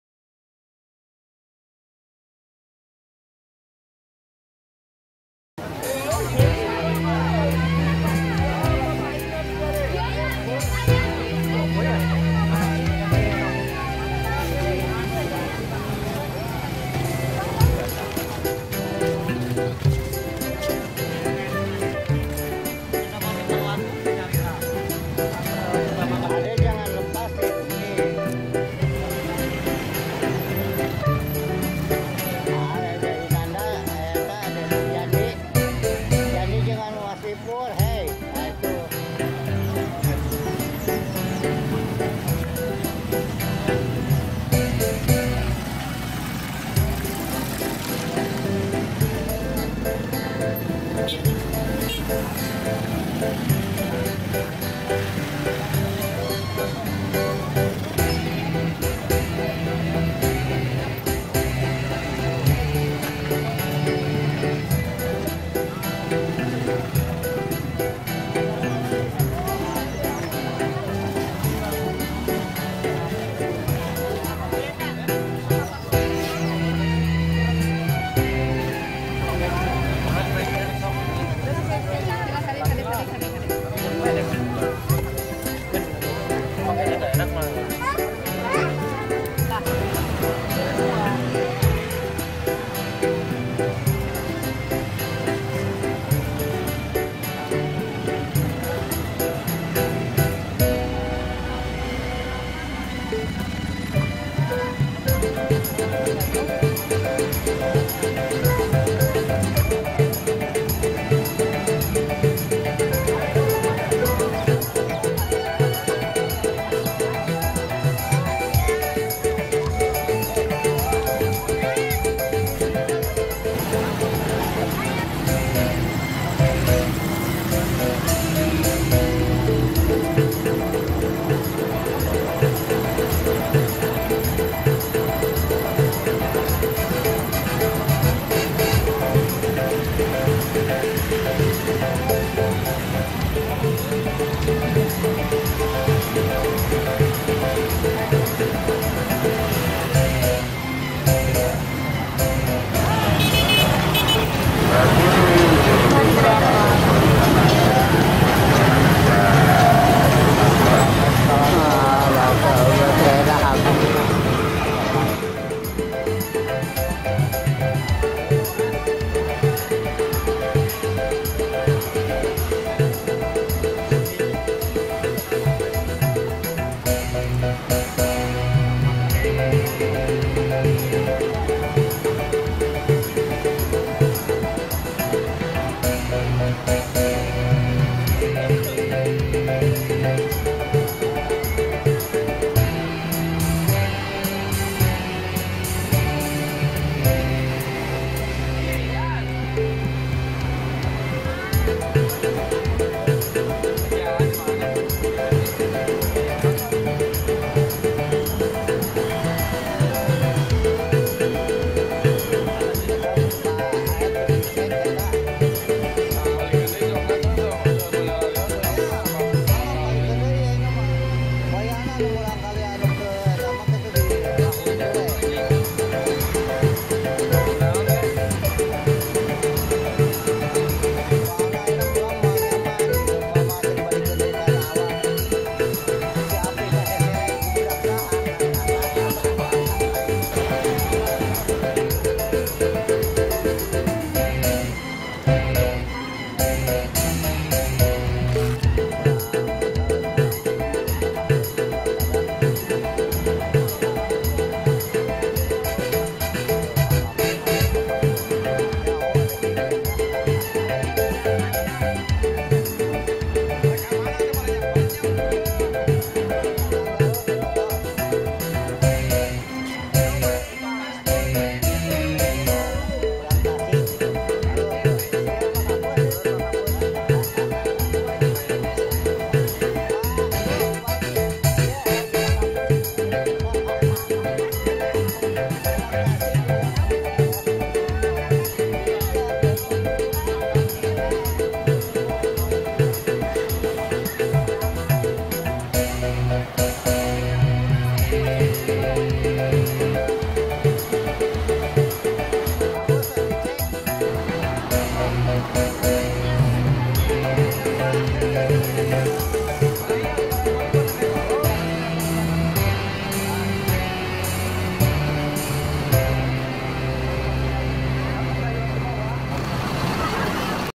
Roads in Malioboro: Vid. 11)) Family Gathering# PGRI & K3S Haurwangi
Perjalanan Wisata Yogyakarta "Family Gathering" PGRI & K3S Kecamatan Haurwangi Cianjur, 26 - 28 Desember 2015